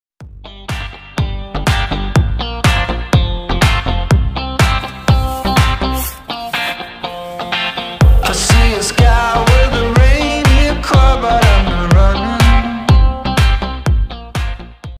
Mustang pizza baker for the gas grill!
Do you wish to make the world’s best pizza on your own yard? The Mustang baker is the optimal accessory for your gas grill. For crispy pizza bases and different types of bread. The pizza oven can also be used for roasting meat and vegetables or cooking fish.